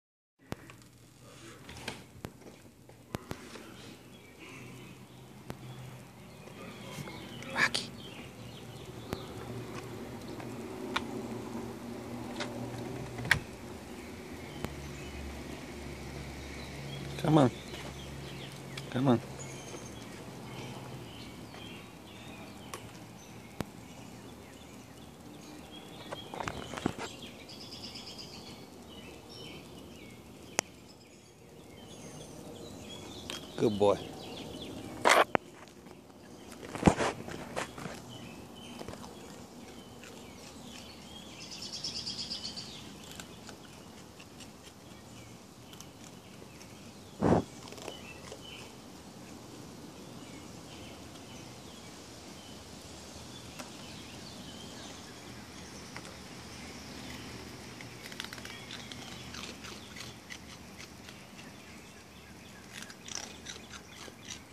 What Could Go Wrong
Trying to feed a wild raccoon with bare hands, great idea.
I was surprised what this raccoon did.

I didn't get mobbed by raccoons.
raccoon loves donuts.